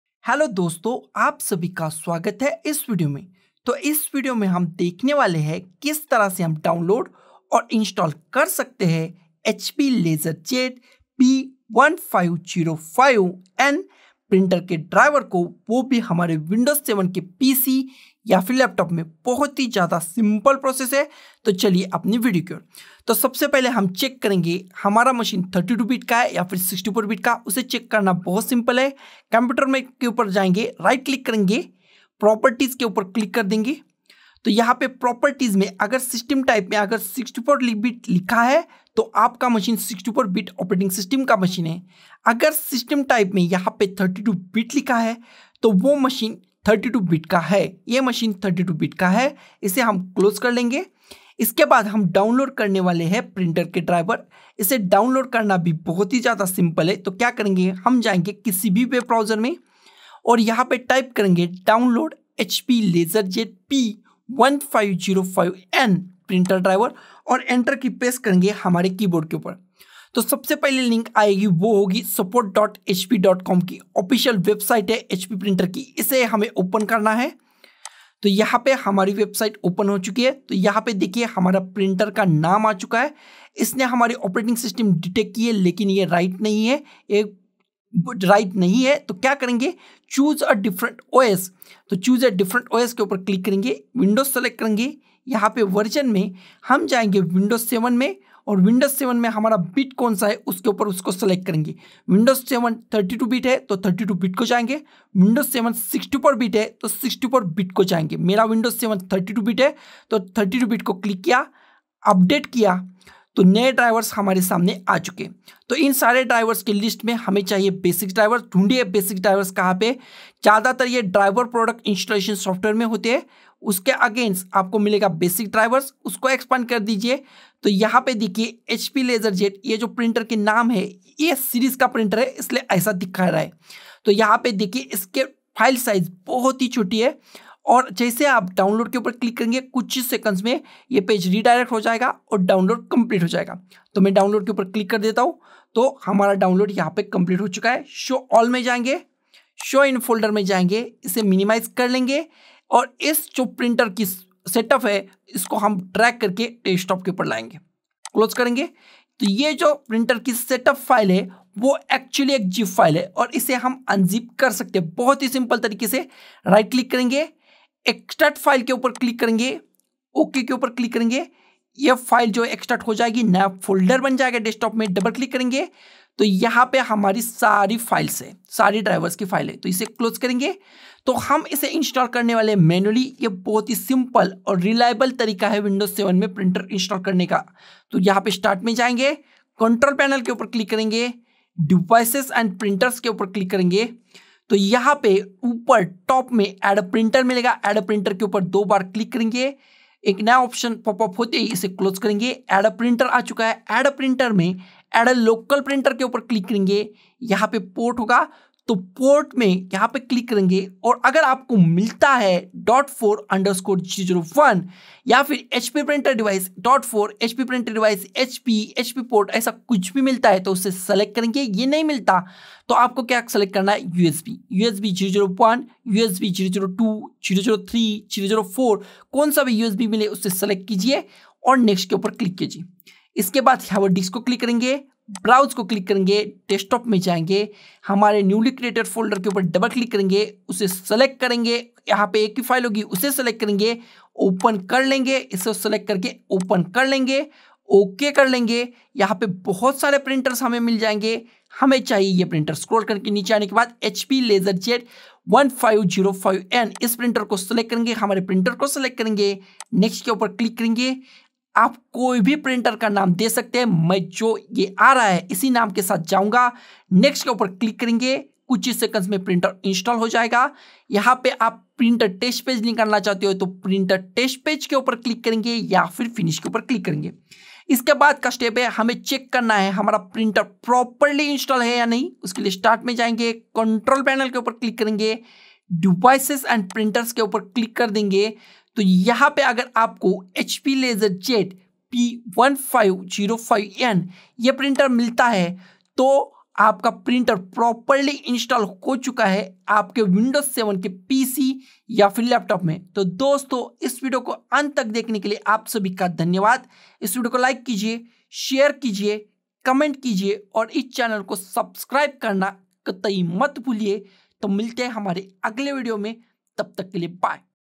How to Download & Install HP Laserjet P1505n Printer Driver in Windows 7 PC or Laptop - Hindi
Welcome to our detailed tutorial on how to manually download and install the HP LaserJet P1505n printer driver on your Windows 7 PC or laptop. Follow this step-by-step guide to ensure your HP LaserJet P1505n is set up and working flawlessly.

🚀 Timestamps:
0:00 Introduction
0:21 Check Windows 7 PC or laptop is 32 bit or 64 bit
0:51 Download HP LaserJet P1505n Printer Driver
2:44  Extracting the Driver Files
3:07 Install HP LaserJet P1505n  Printer Driver in Windows 7 PC or laptop
4:42 Check Printer is Properly installed or not in Windows 7
5:03 Conclusion

📋 Steps Covered in This Video:

Open your web browser and go to the HP Support website.
Search for the HP LaserJet P1505n printer driver compatible with Windows 7.
Download the appropriate driver file.

Extracting the Driver Files:
Locate the downloaded file in your Downloads folder.
Right-click the file and select "Extract here" or "Extract to [folder name]."

Installing the Driver Manually:
Open the Control Panel and go to "Devices and Printers."
Click on "Add a printer" and select "Add a local printer."
Choose "Use an existing port" and select the appropriate port.
Click "Have Disk" and browse to the location where you extracted the driver files.
Select the driver file and complete the installation.

Verifying the Installation:
Open "Devices and Printers" to ensure the HP LaserJet P1505n is listed.
Print a test page to verify the printer is working correctly.